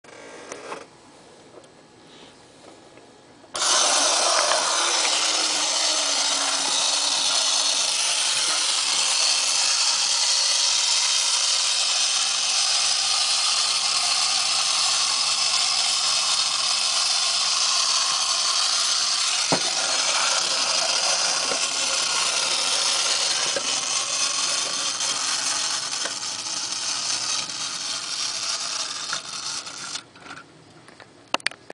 Mechanical Action Planet Robot Tin Wind-up Toy Red
Presenting a brand new reproduction red Mechanical Planet Robot from Schylling Toys.   This is a great tin toy reproduction! It looks good, its plays good and it even feels good! The detailed graphics invite you to take a closer look.

As he walks sparks fly inside his face mask! On/Off switch and Key.

Measures 10 3/4" inches tall.

Can be played with or looks great on display!